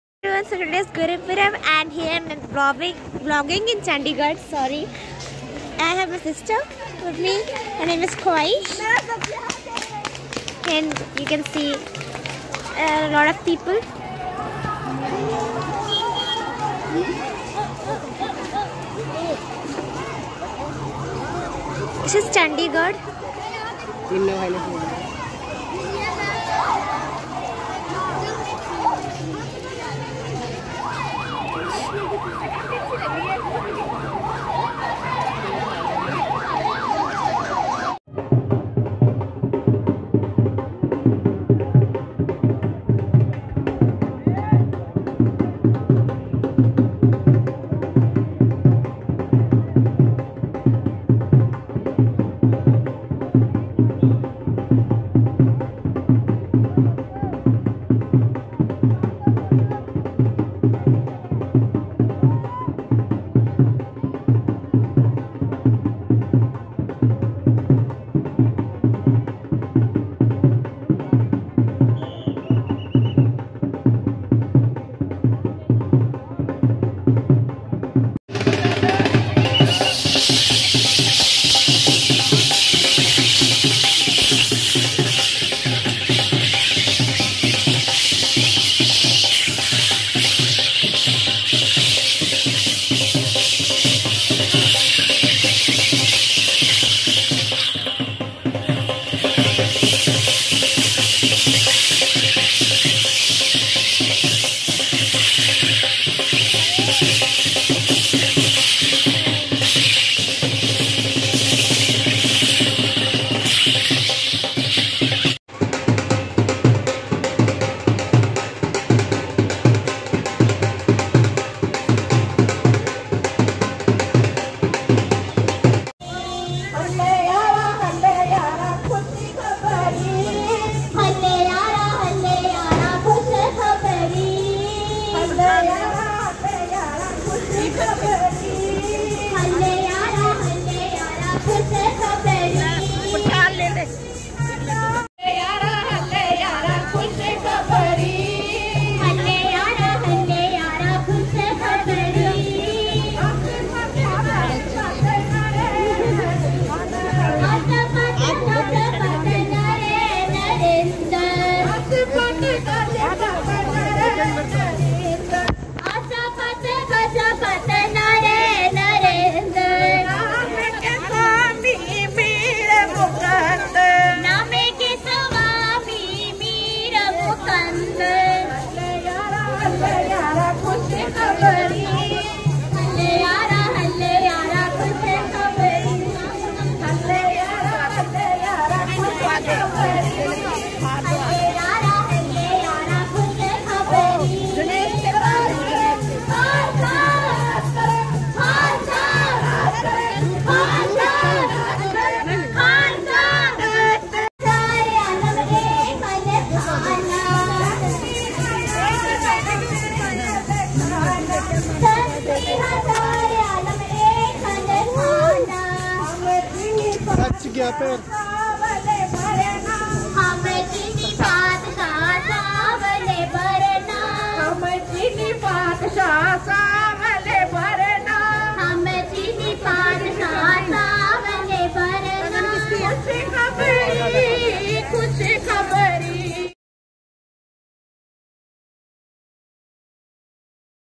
Gurupurab Nagar Kirtan Chandigarh 2021 - Chandigarh vlog
Anyways This is the Nagar Kirtan of Chandigarh
This was my first time seeing anything like this , Hope you guys Like this